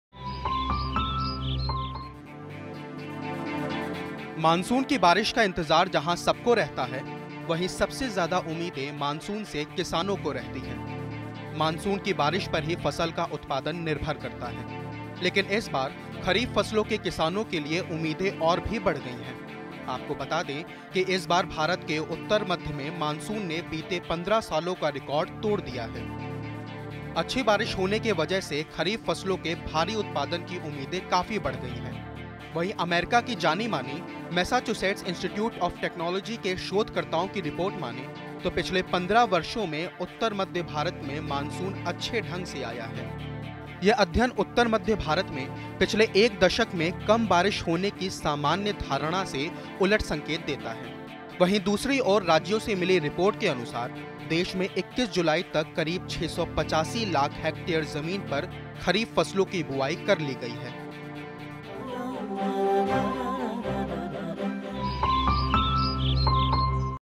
Kharif फसलों के लिए Monsoon पिछले 10 सालो से बेहतर
पिछले 10 सालो से बेहतर  मॉनसून के कारण खरीफ फसलों की बम्पर उत्पादन का अंदाज़ा।